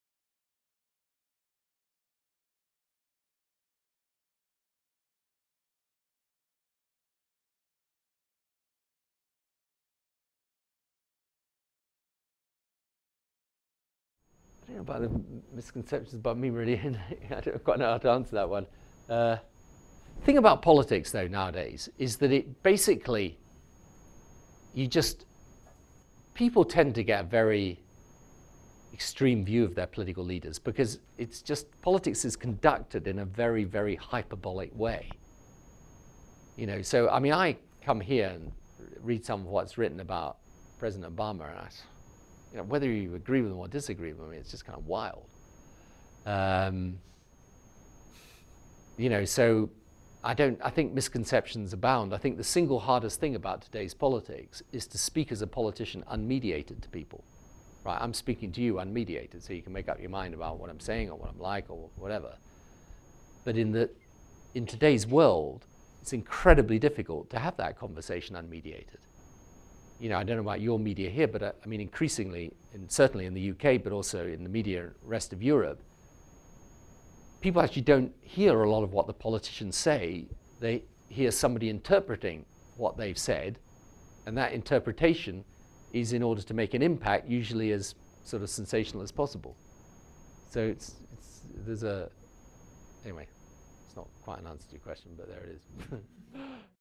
Tony Blair: Misconceptions
Tony Blair, former Prime Minister of Britain, founder and patron of the Tony Blair Faith Foundation, discusses misconceptions about political leaders.